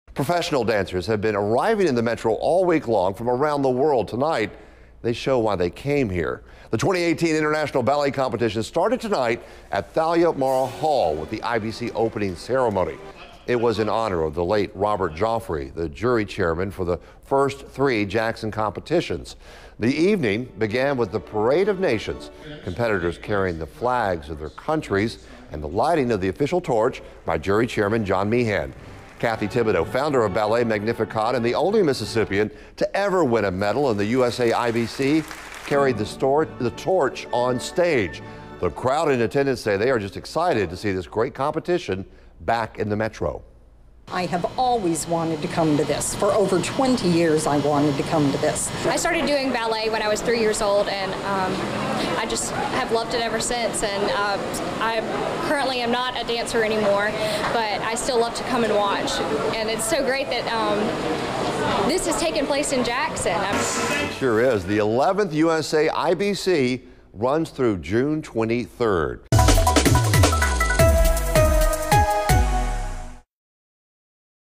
IBC commences with opening ceremonies
The 2018 International Ballet Competition started tonight at Thalia Mara Hall with the its opening ceremonies.